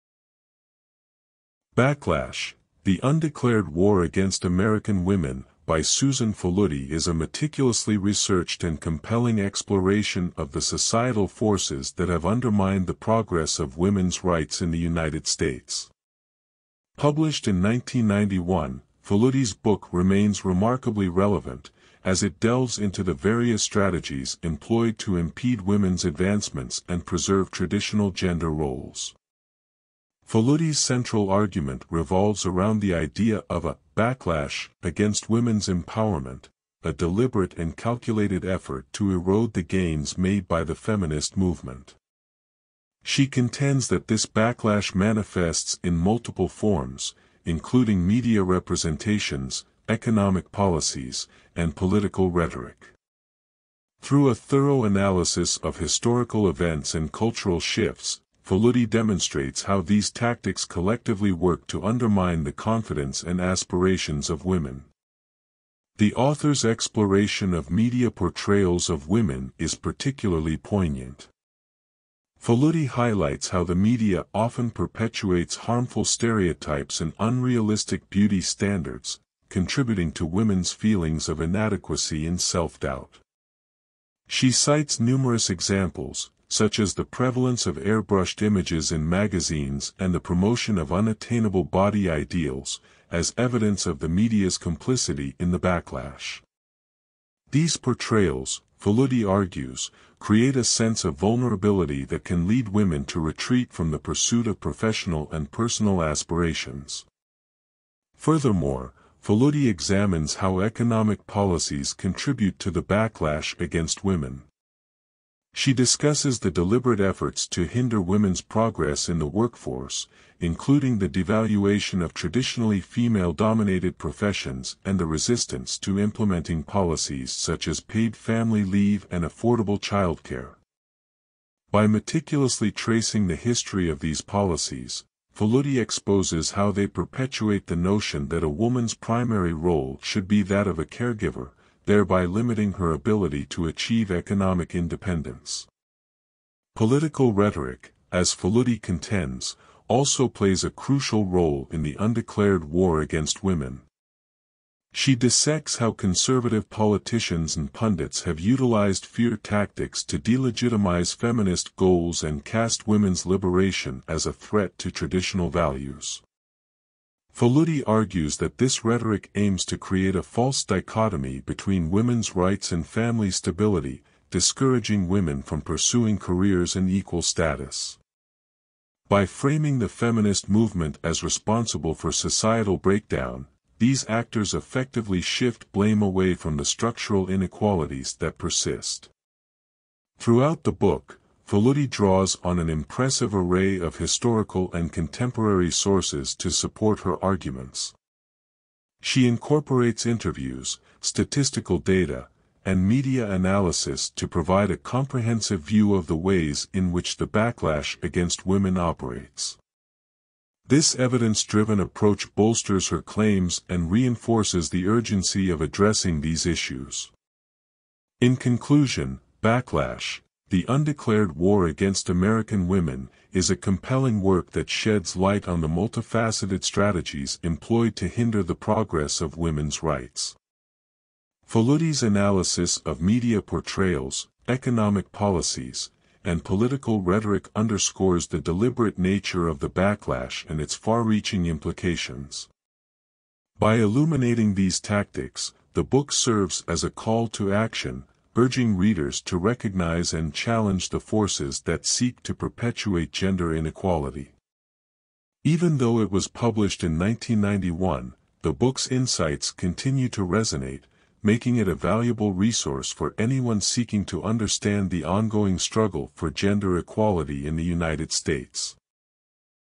"Backlash" By Susan Faludi
"Backlash: The Undeclared War Against American Women" by Susan Faludi is a meticulously researched and compelling exploration of the societal forces that have undermined the progress of women's rights in the United States. Published in 1991, Faludi's book remains remarkably relevant, as it delves into the various strategies employed to impede women's advancements and preserve traditional gender roles.

Faludi's central argument revolves around the idea of a "backlash" against women's empowerment – a deliberate and calculated effort to erode the gains made by the feminist movement. She contends that this backlash manifests in multiple forms, including media representations, economic policies, and political rhetoric. Through a thorough analysis of historical events and cultural shifts, Faludi demonstrates how these tactics collectively work to undermine the confidence and aspirations of women.

The author's exploration of media portrayals of women is particularly poignant. Faludi highlights how the media often perpetuates harmful stereotypes and unrealistic beauty standards, contributing to women's feelings of inadequacy and self-doubt. She cites numerous examples, such as the prevalence of airbrushed images in magazines and the promotion of unattainable body ideals, as evidence of the media's complicity in the backlash. These portrayals, Faludi argues, create a sense of vulnerability that can lead women to retreat from the pursuit of professional and personal aspirations.

Furthermore, Faludi examines how economic policies contribute to the backlash against women. She discusses the deliberate efforts to hinder women's progress in the workforce, including the devaluation of traditionally female-dominated professions and the resistance to implementing policies such as paid family leave and affordable childcare. By meticulously tracing the history of these policies, Faludi exposes how they perpetuate the notion that a woman's primary role should be that of a caregiver, thereby limiting her ability to achieve economic independence.

Political rhetoric, as Faludi contends, also plays a crucial role in the undeclared war against women. She dissects how conservative politicians and pundits have utilized fear tactics to delegitimize feminist goals and cast women's liberation as a threat to traditional values. Faludi argues that this rhetoric aims to create a false dichotomy between women's rights and family stability, discouraging women from pursuing careers and equal status. By framing the feminist movement as responsible for societal breakdown, these actors effectively shift blame away from the structural inequalities that persist.

Throughout the book, Faludi draws on an impressive array of historical and contemporary sources to support her arguments. She incorporates interviews, statistical data, and media analysis to provide a comprehensive view of the ways in which the backlash against women operates. This evidence-driven approach bolsters her claims and reinforces the urgency of addressing these issues.

In conclusion, "Backlash: The Undeclared War Against American Women" is a compelling work that sheds light on the multifaceted strategies employed to hinder the progress of women's rights. Faludi's analysis of media portrayals, economic policies, and political rhetoric underscores the deliberate nature of the backlash and its far-reaching implications. By illuminating these tactics, the book serves as a call to action, urging readers to recognize and challenge the forces that seek to perpetuate gender inequality. Even though it was published in 1991, the book's insights continue to resonate, making it a valuable resource for anyone seeking to understand the ongoing struggle for gender equality in the United States.